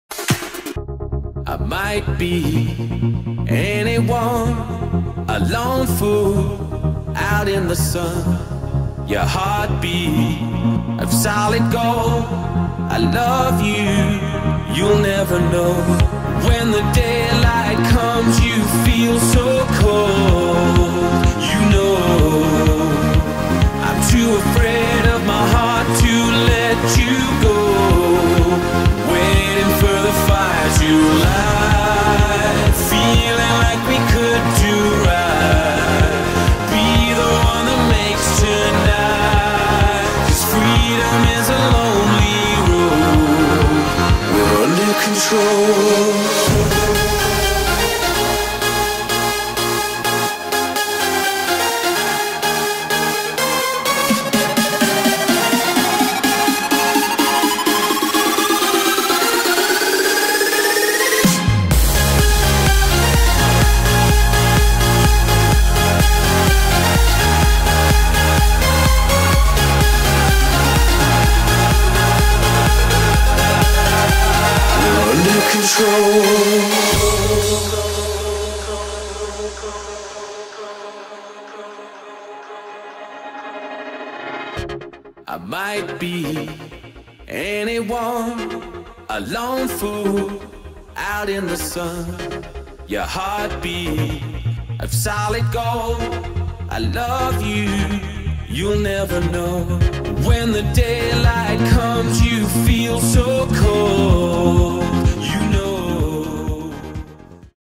DHC vs StAidens 3 12 13
Brief highlights of the National School's Basketball League game between Dyke House Sports and Technology College and St. Aiden's Academy.